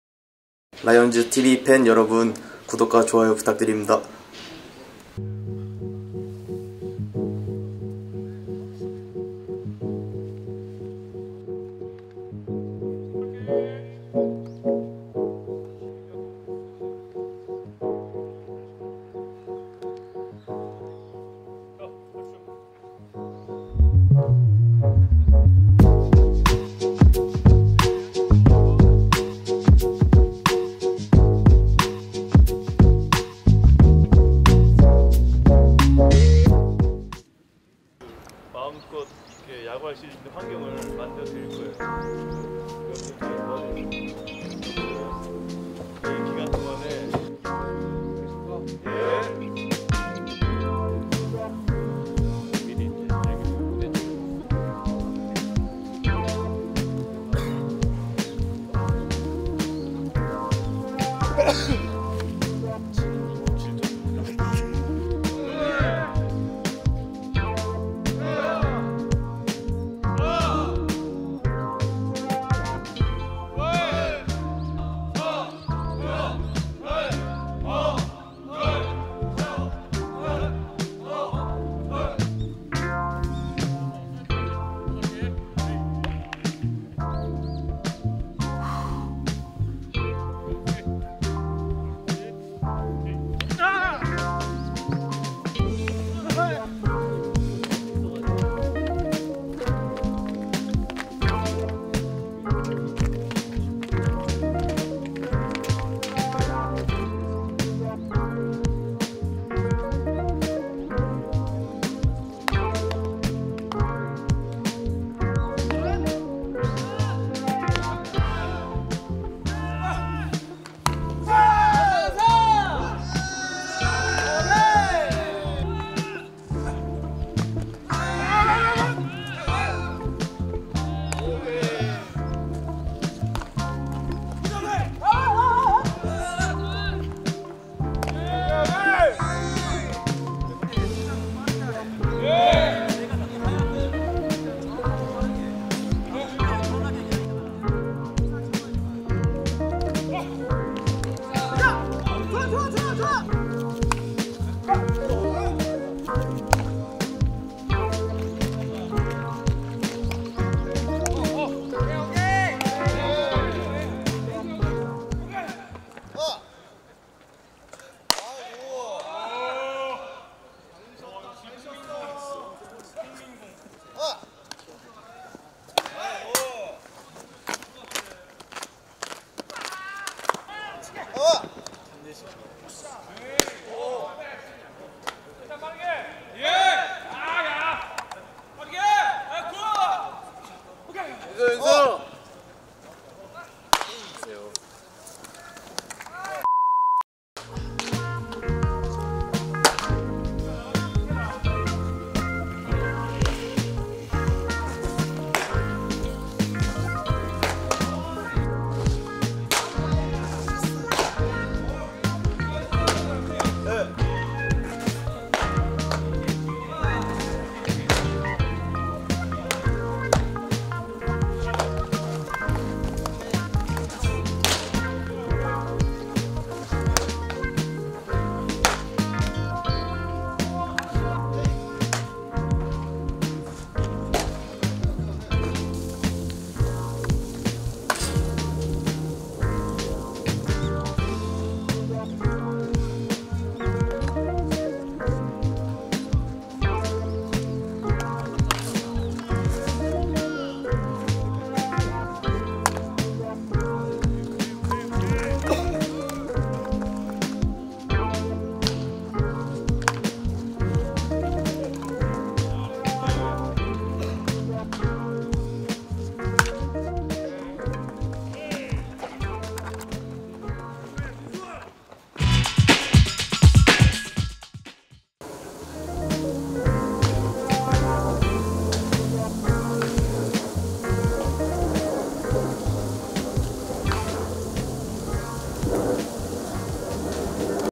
[라이온즈tv] 훈련스케치 🏋 마무리 훈련 시작
마무리 훈련 첫날,
경산에서 다음 시즌 준비를 시작한 선수단을 만났습니다.
몸을 풀어준 뒤 바로 훈련에 돌입한 선수들,
🍂 낙엽비가 🍁 내리는 모습도 절경이네요 장관이구요.